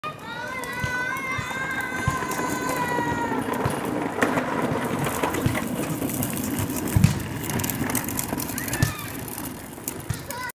Need for speed
Dominic racing with his Bobby Car in our garden